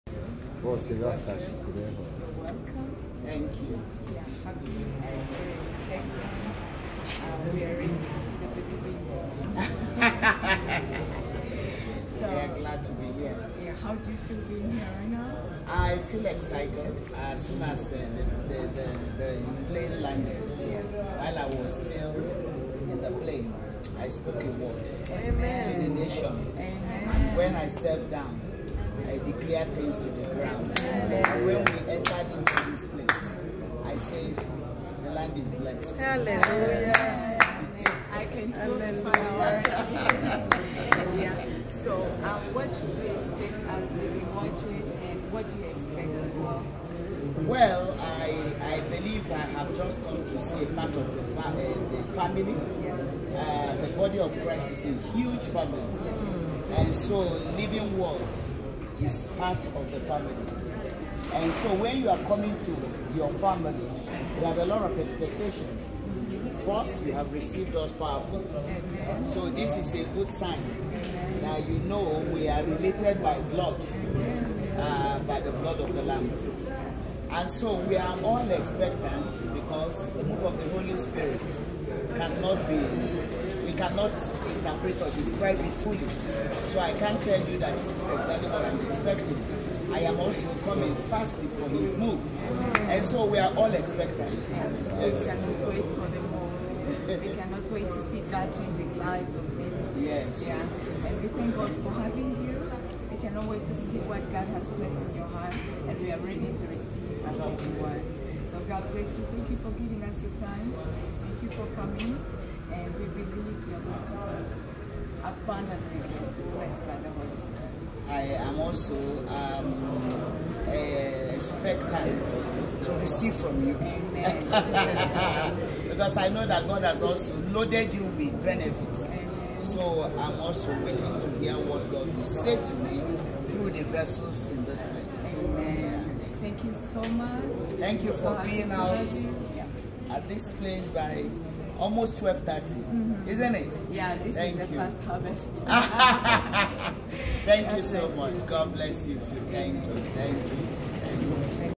Arrival at Kigali Airport
Arrival at Kigali Airport. Thanks to our host Bishop & Mrs  Karamera Emmanuel and team for a warm kingdom reception and interview on arrival. They eagerly waited until we came out at 12.45am.

 We are assured of God's blessings this week in Jesus Name. Wherever you are in Rwanda please purpose to attend the Queen Esthers' Generation Rwanda Launch at Living Word Church. It's time for the manifestation of God's people.